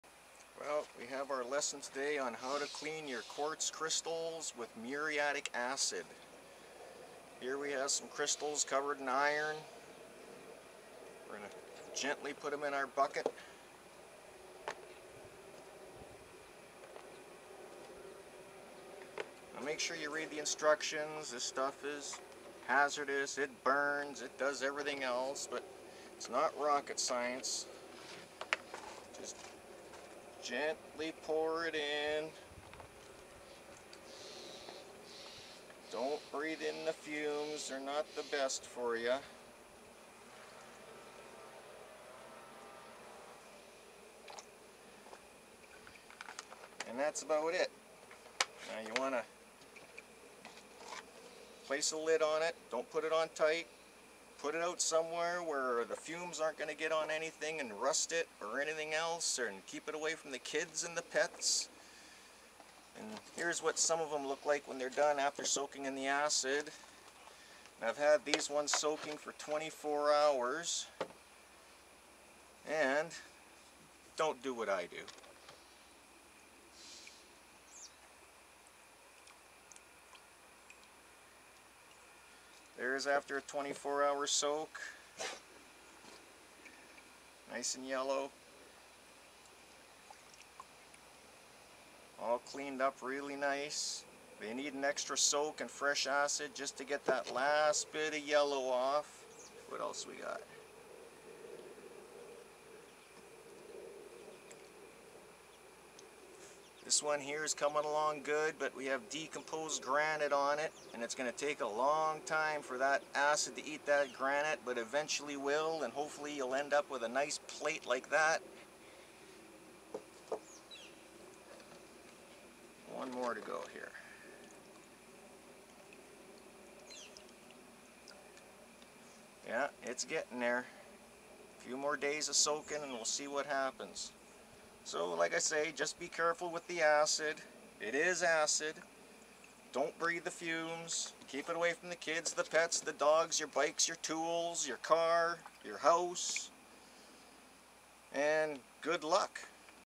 cleaning quartz crystals
cleaning quartz crystals with muriatic acid,folks this is acid so do not get it on you or in you do not breath the fumes,do it in an open area away from pets,children,metal,cars,trucks your house,if you do screw it up keep some baking soda mixed with water to neutralize the acid and when you are done cleaning your crystals give them a rinse to get rid of the acid left on them,be safe and have fun